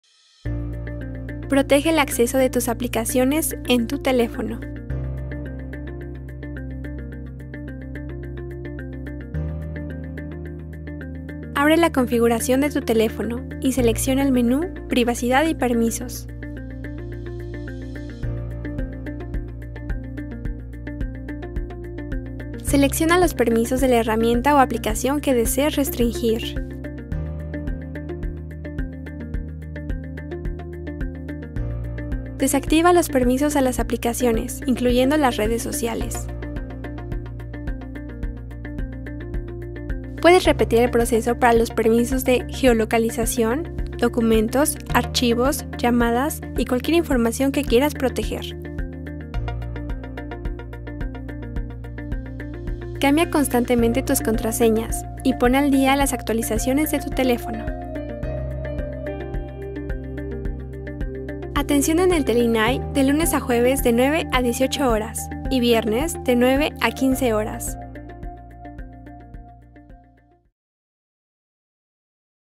Protege el acceso a las apps de tu teléfono
Así puedes controlar el acceso que las apps puedan tener a tus fotos, contactos y otros datos sensibles.